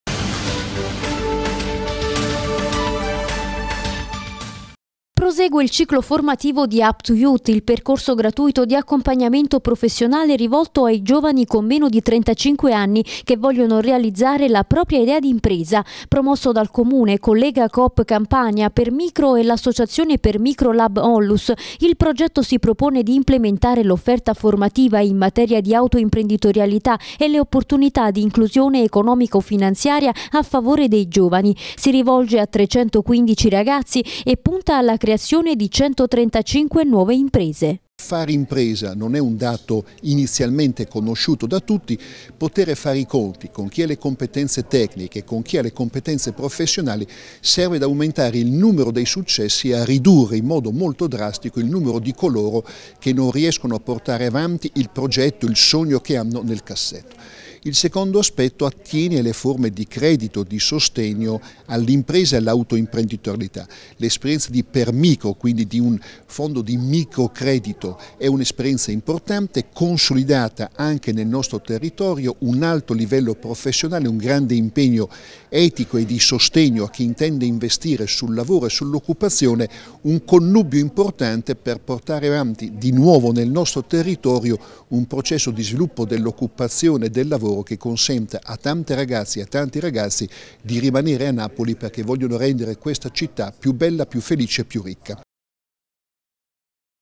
Giovani e impresa, la formazione continua
Prosegue il ciclo formativo di Up to Youth, percorso di accompagnamento professionale rivolto ai giovani che vogliono realizzare un'idea di impresa.